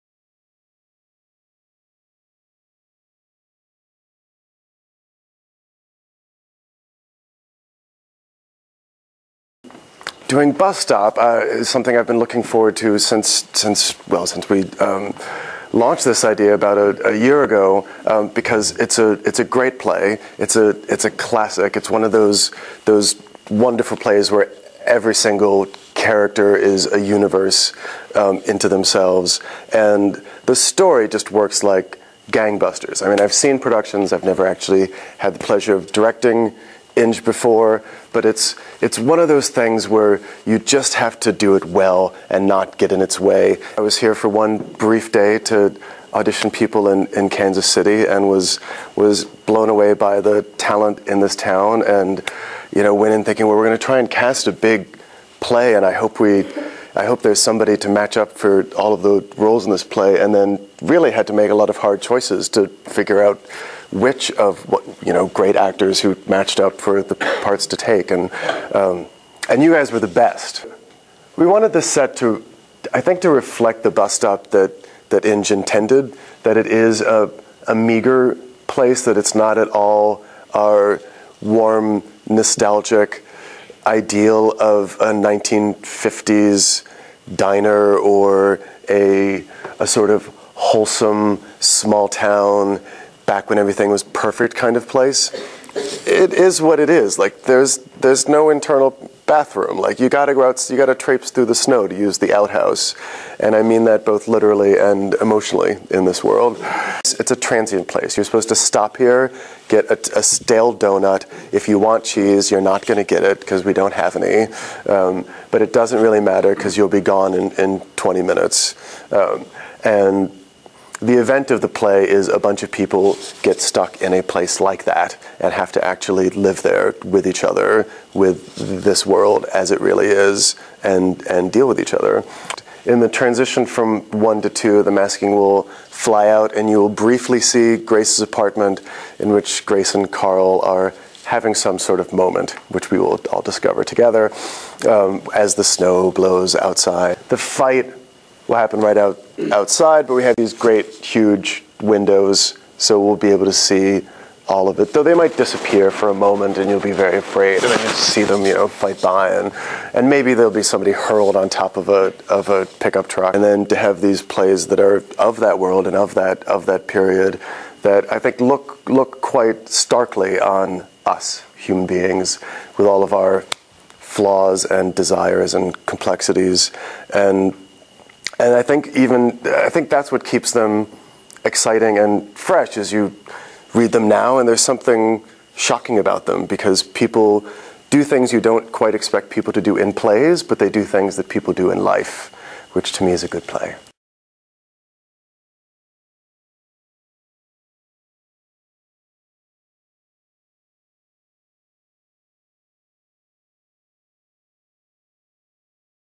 Steven Cosson talks about BUS STOP
Director Steven Cosson talks about his vision for the Kansas City Repertory Theatre production of BUS STOP. The show is at the Rep's Spencer Theatre and runs March 12 - April 3, 2010. The show's artistic team includes: Andromache Chalfant (sets), Sarah Beers (Costumes) David Weiner (Lighting), Joe Cerqua (Sound), Jan DeLovage (Wigs) and Mary Honour (Stage Manager). The cast includes: Blair Baker (Elma) David Fritts (Carl), Jim Gall (Will), Gary Neal Johnson (Virgil), Mark Robbins (Dr. Lyman), Jedadiah Schultz (Bo) Adria Vitler (Cherie) and Cheryl Weaver (Grace).